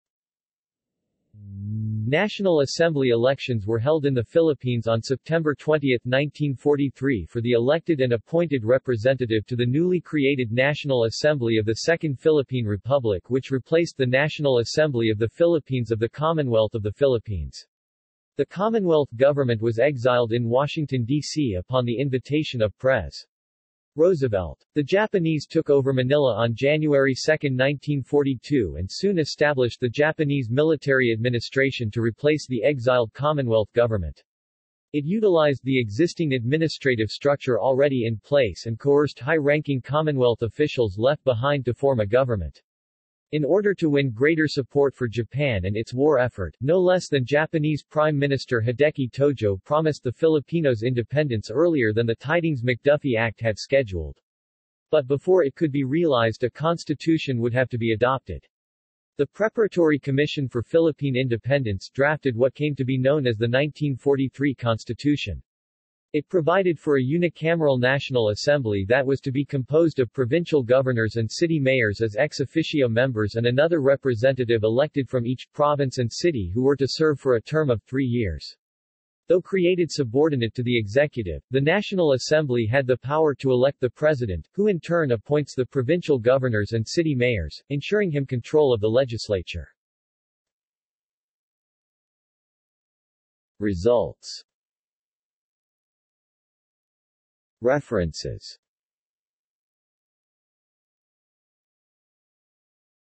Philippine legislative election, 1943 | Wikipedia audio article
This is an audio version of the Wikipedia Article:
Philippine legislative election, 1943

SUMMARY
National Assembly elections were held in the Philippines on September 20, 1943 for the elected and appointed representative to the newly created National Assembly of the Second Philippine Republic which replaced the National Assembly of the Philippines of the Commonwealth of the Philippines. The Commonwealth government was exiled in Washington, D.C. upon the invitation of Pres. Roosevelt. The Japanese took over Manila on January 2, 1942 and soon established the Japanese Military Administration to replace the exiled Commonwealth government. It utilized the existing administrative structure already in place and coerced high-ranking Commonwealth officials left behind to form a government. In order to win greater support for Japan and its war effort, no less than Japanese Prime Minister Hideki Tōjō promised the Filipinos independence earlier than the Tydings-McDuffie Act had scheduled. But before it could be realized a constitution would have to be adopted. The Preparatory Commission for Philippine Independence drafted what came to be known as the 1943 Constitution. It provided for a unicameral National Assembly that was to be composed of provincial governors and city mayors as ex officio members and another representative elected from each province and city who were to serve for a term of three years. Though created subordinate to the executive, the National Assembly had the power to elect the President, who in turn appoints the provincial governors and city mayors, ensuring him control of the legislature.